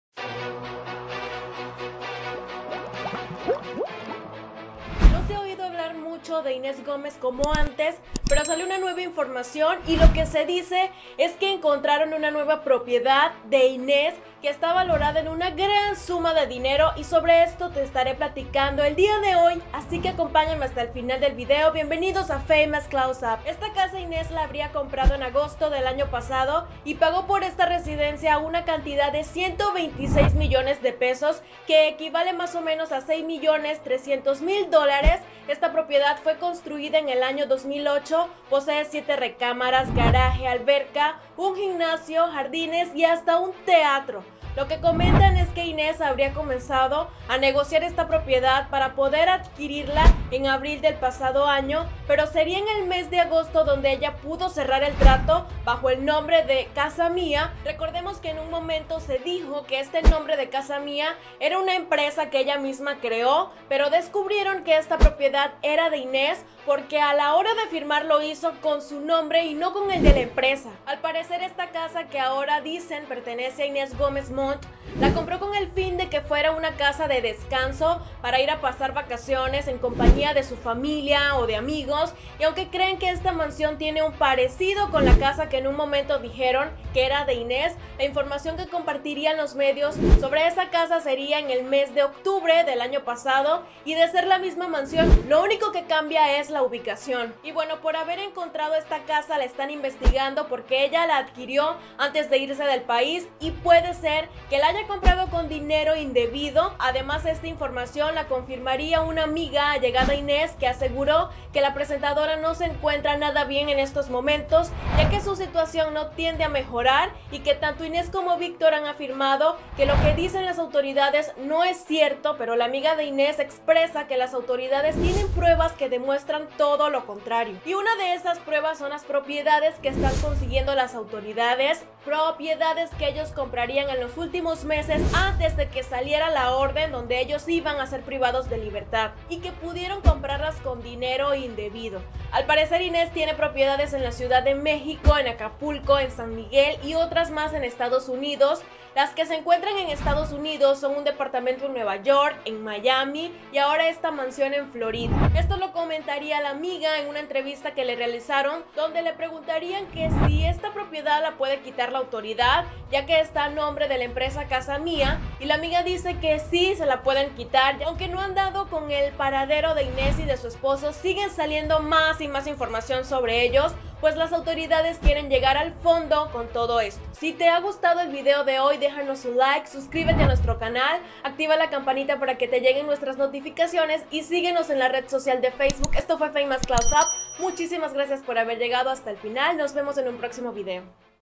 🔴📣 𝗡𝗨𝗘𝗩𝗔 𝗬 𝗟𝗨𝗝𝗢𝗦𝗔 mansión de Inés Mont que vale 126 millones de pesos
Revelan detalles de otra casa encontrada a nombre de In3s Gómez Mont…